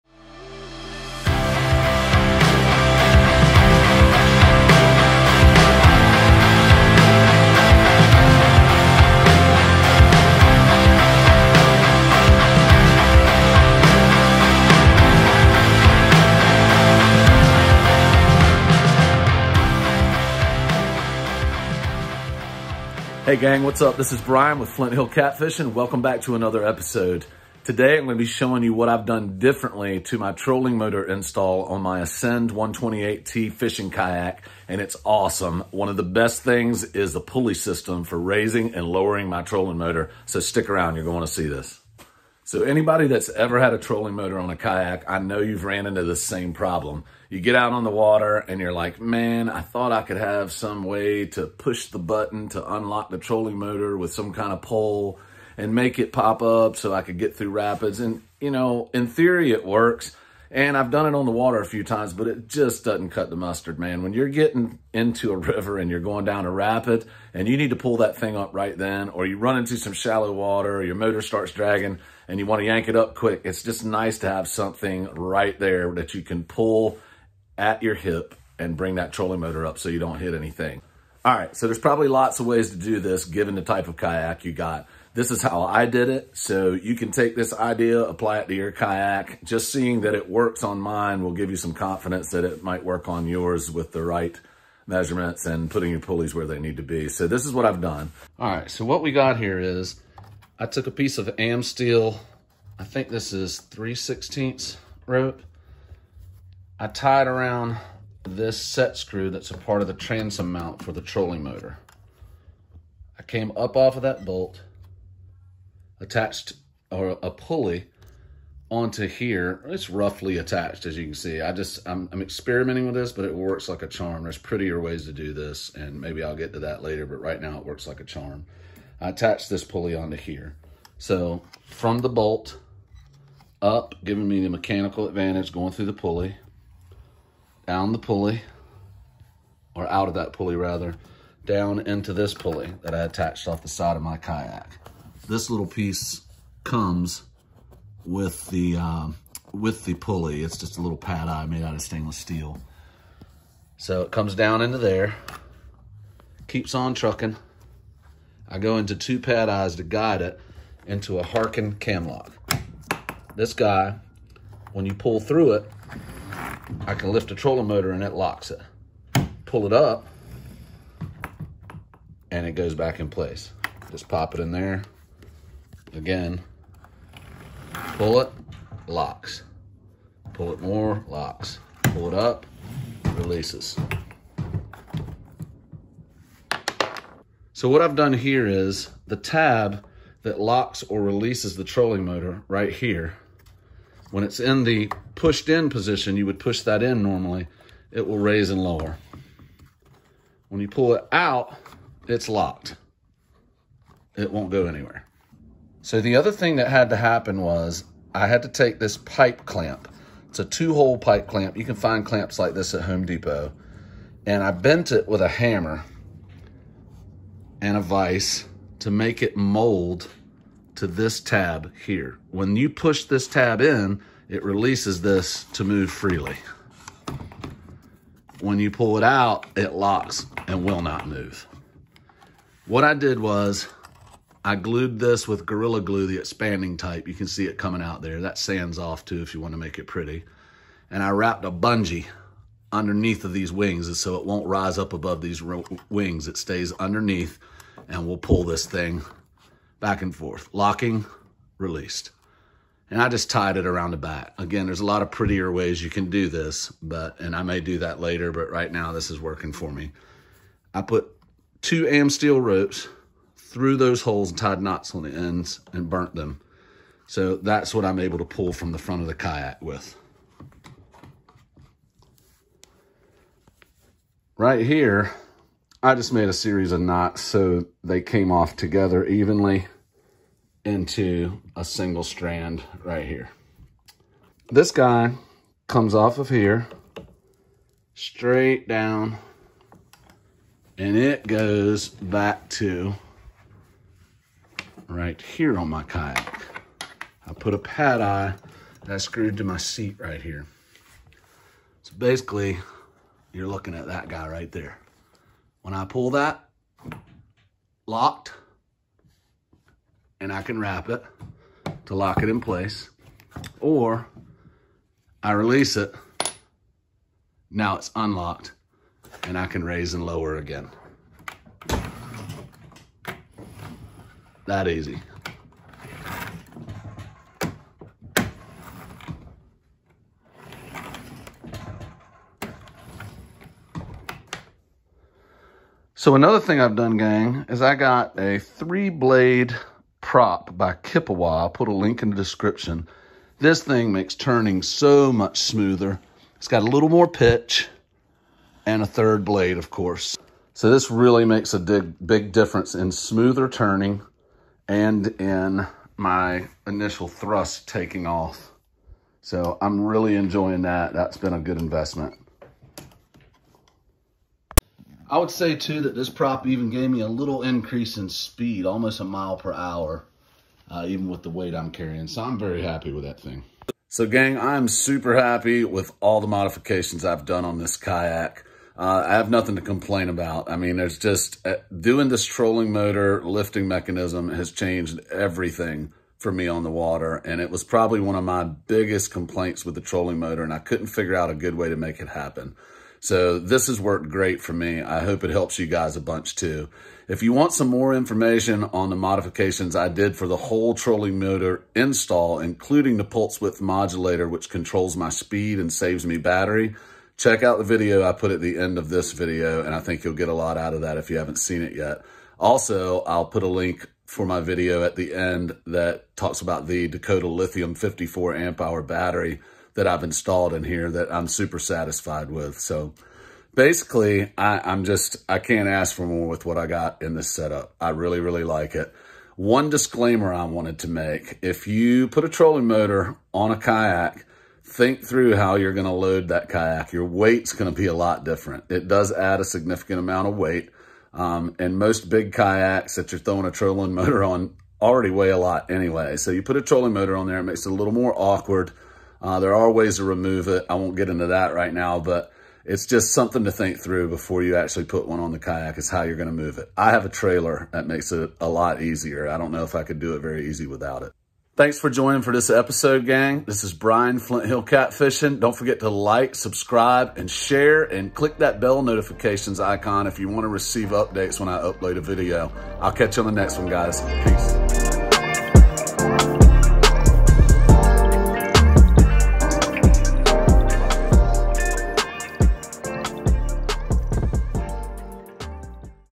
Killer Mod to Easily Raise and Lower a Trolling Motor on your Fishing Kayak
I recently installed a pulley system to raise and lower my trolling motor on my Ascend 128T fishing kayak and I LOVE it!!! I think you will too when you see how easy it is to install. This is by far one of the most useful mods that I've installed on my kayak. Check it out!!

🔥Here are the links to put this system together🔥

AMSTEEL ROPE

Samson Amsteel Blue 7/64" 100',...

HARKEN MICRO CAM LOCK

HARKEN Micro Carbo-Cam Cleat -...

HARKEN PULLEYS

HARKEN 443 Micro 22mm...

KIPIWA PROP

Kipawa 314 High Performance...